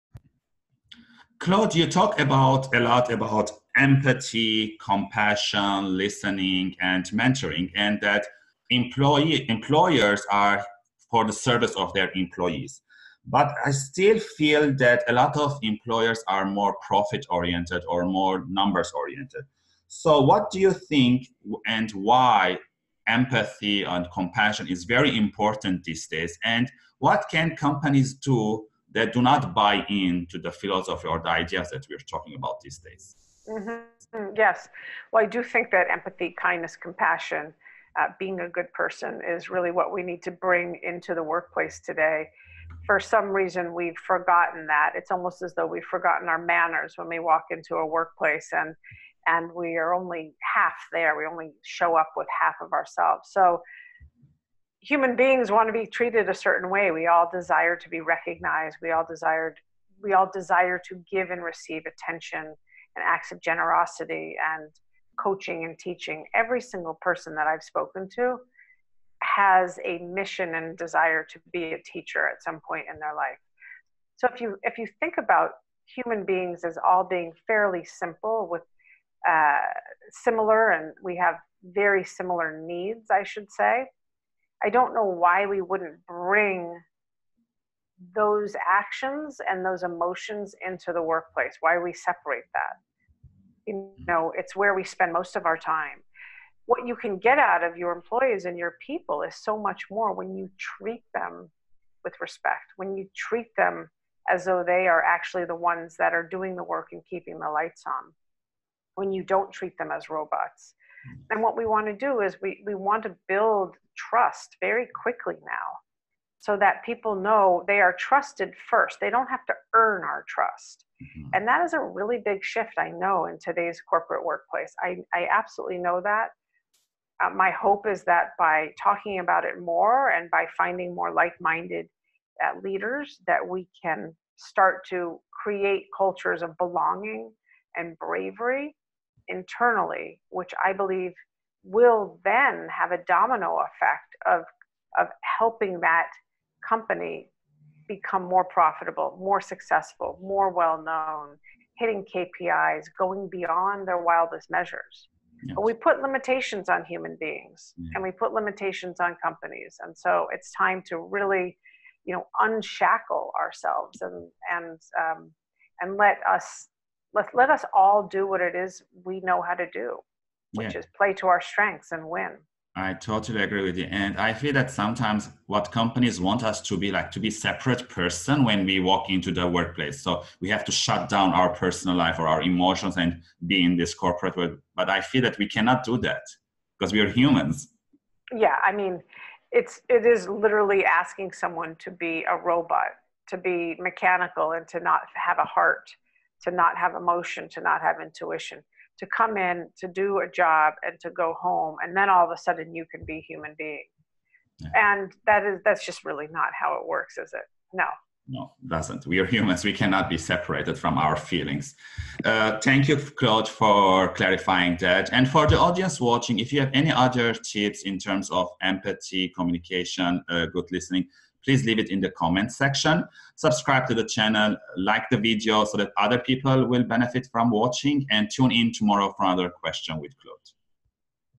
Empathy at Workplace
For some reason, we've forgotten that it's almost as though we've forgotten our manners when we walk into a workplace and, and we are only half there.  We only show up with half of ourselves. So, human beings want to be treated a certain way, we all desire to be recognized, we all desired desire to give and receive attention, and acts of generosity and coaching and teaching. Every single person that I've spoken to, has a mission and desire to be a teacher at some point in their life.

So if you think about human beings, as all being fairly simple with similar and we have very similar needs, I should say. I don't know why we wouldn't bring those actions and those emotions into the workplace, why we separate that? It's where we spend most of our time, what you can get out of your employees and your people is so much more when you treat them with respect. When you treat them as though they are actually the ones that are doing the work and keeping the lights on when you don't treat them as robots.

What we want to do is, we want to build trust very quickly now. So, that people know they are trusted first, they don't have to earn our trust. That is a really big shift, I know in today's corporate workplace, I absolutely know that.

My hope is that by talking about it more, and by finding more like minded leaders, that we can start to create cultures of belonging and bravery, internally, which I believe, will then have a domino effect of helping that company become more profitable, more successful, better known, hitting KPIs going beyond their wildest measures.

We put limitations on human beings, and we put limitations on companies and so it's time to really, you know, unshackle ourselves and let us let us all do what it is we know how to do, which is play to our strengths and win.

It's It is literally asking someone to be a robot to be mechanical and to not have a heart, to not have emotion, to not have intuition, to come in to do a job and to go home and then all of a sudden you can be human being. That is that's just really not how it works, is it? No.